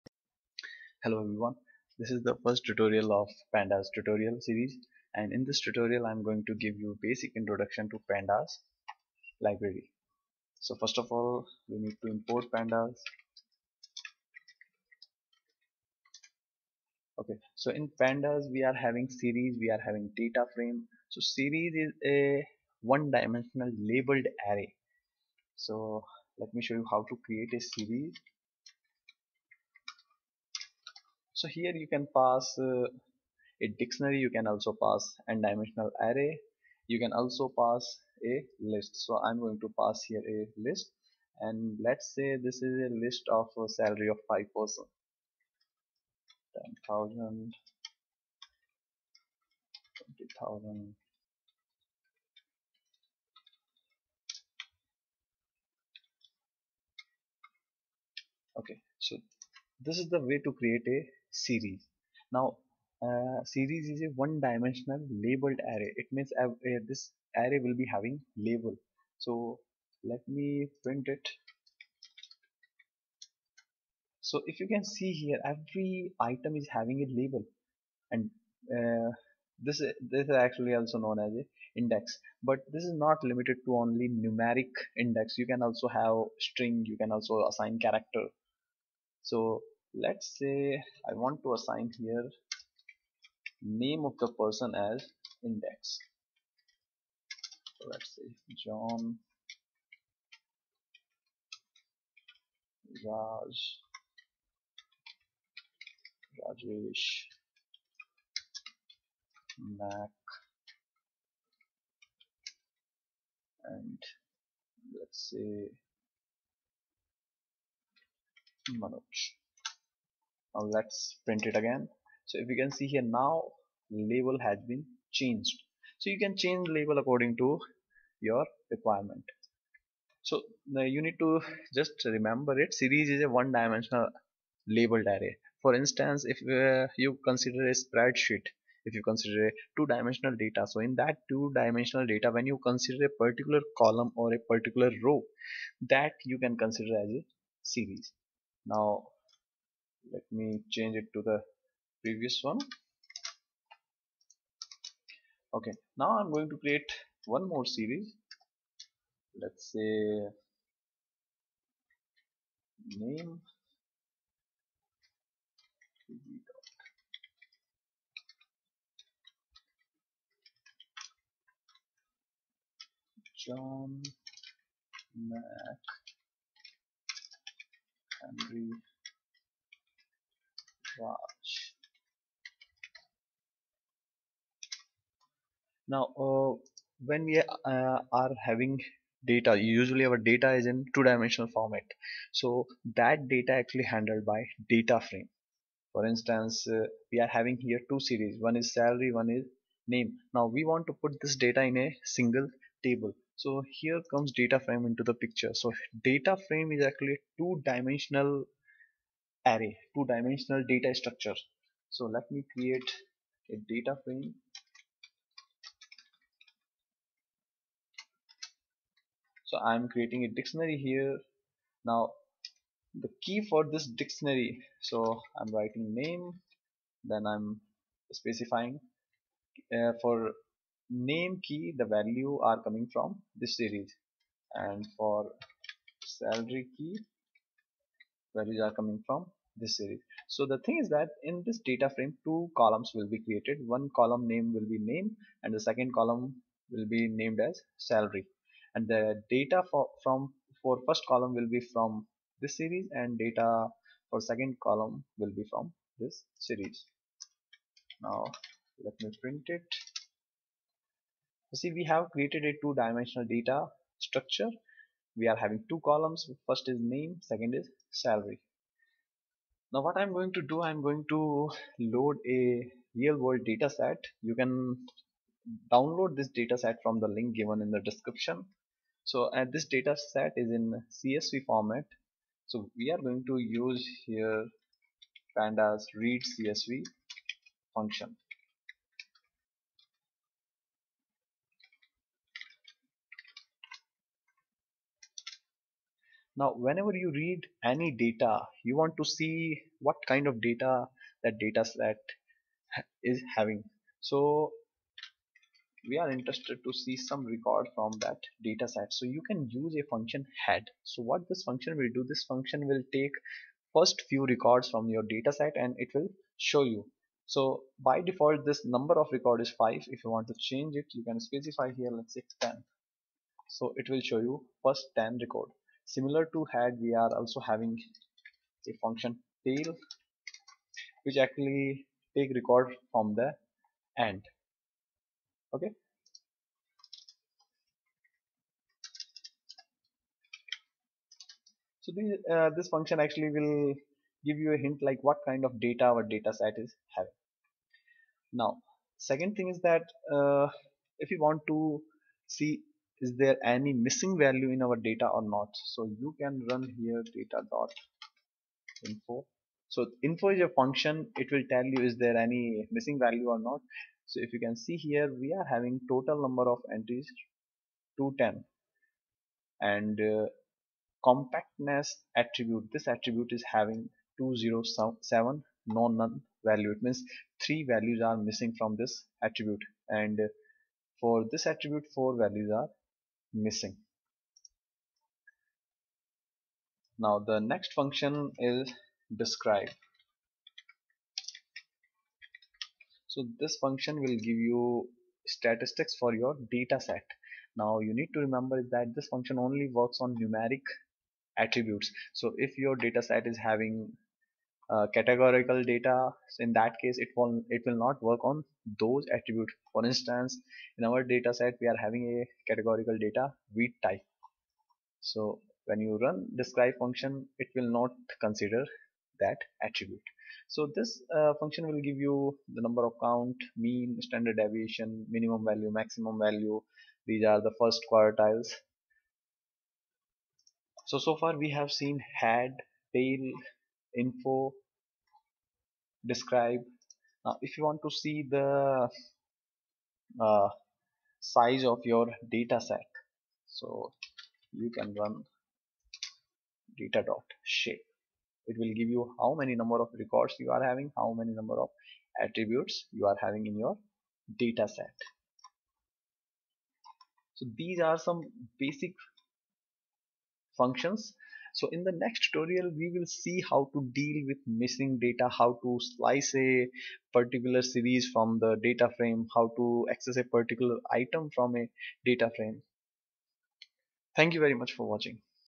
Pandas Tutorials # 1 : Basic Introduction to Pandas
This video will give you basic introduction to Pandas library.